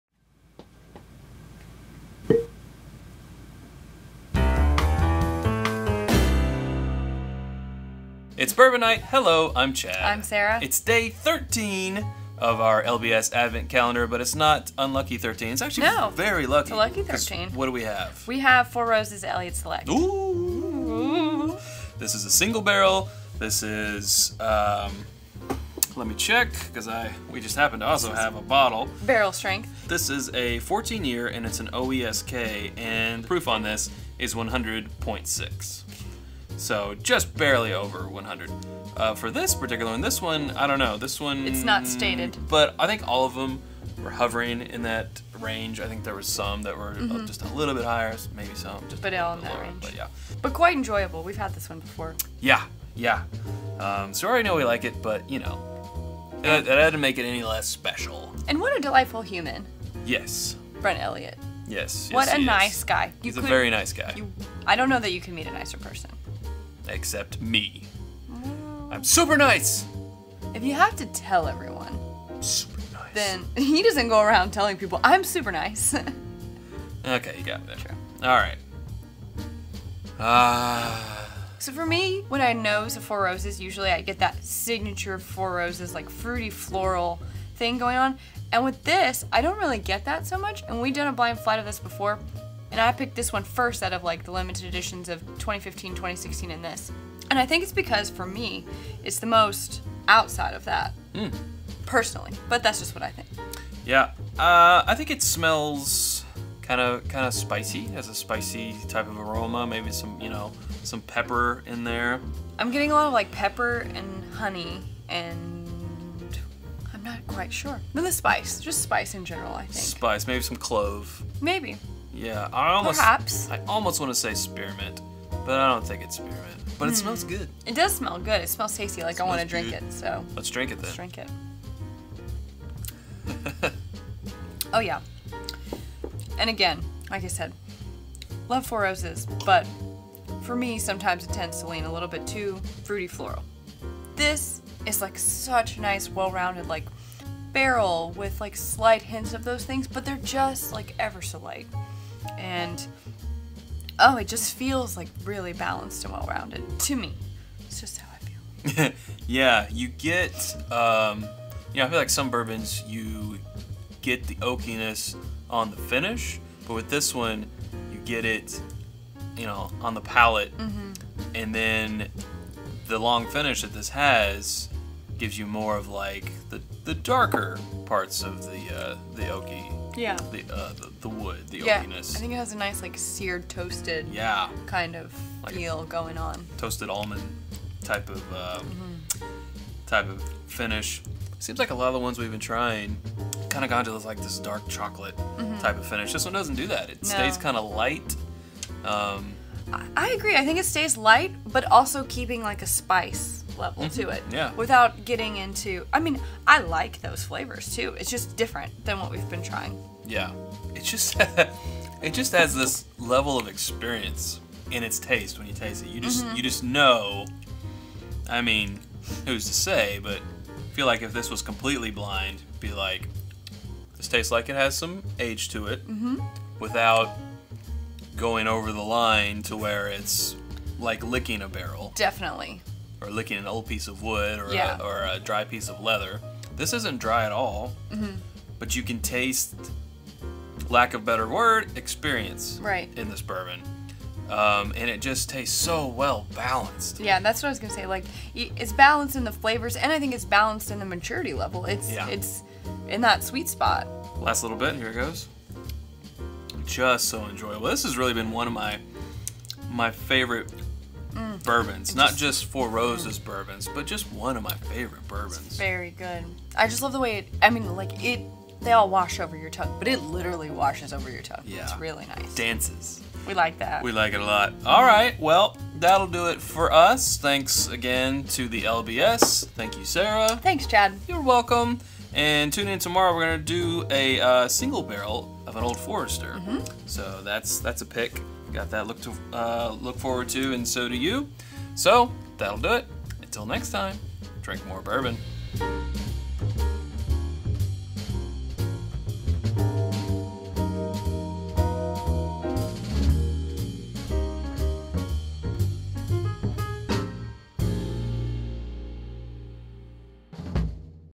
Four Roses Elliot's Select Single Barrel (Advent Calendar Day 13)
Day 13 of the Lexington Bourbon Society Advent Calendar brings us Elliot's Select, a Single Barrel, Barrel Strength Limited Edition from Four Roses.  It's named for Brent Elliot who took over the role as Master Distiller last year.  The recipe is a 14 year old OESK and comes in at 100.6 proof.

Like the glasses and other items we use for Flight Fights,
Uncorkings and more?